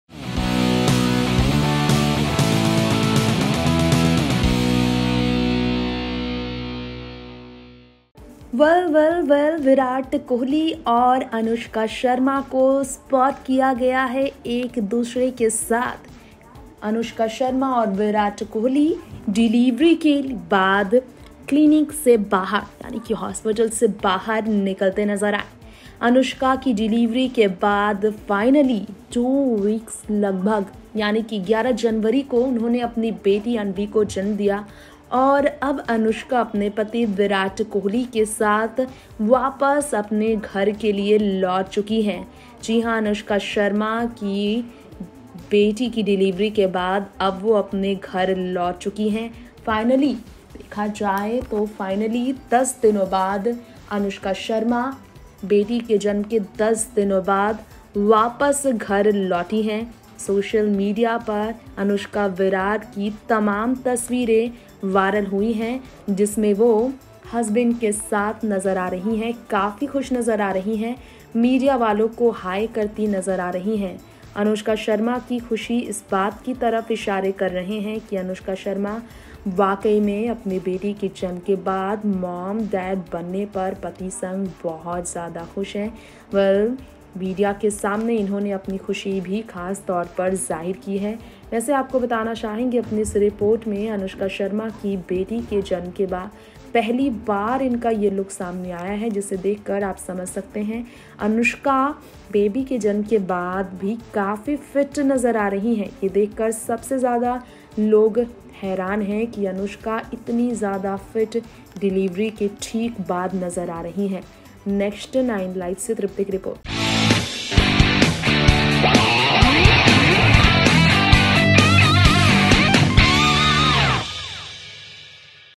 डिलीवरी के 10 दिन बाद घर लौटी अनुष्का शर्मा, मां बनने के बाद भी दिखी इतनी फिट | Anushka Sharma
बेटी के जन्म के 10 दिन बाद आखिरकार अनुष्का शर्मा अपने पति विराट कोहली के साथ लौट आई हैं घर, जिन्हे देखकर यकीन करना कठीन हो रहा हैं कि उन्होने एक बेटी को जन्म दिया हैं..असल में वो इतनी फिट लग रही हैं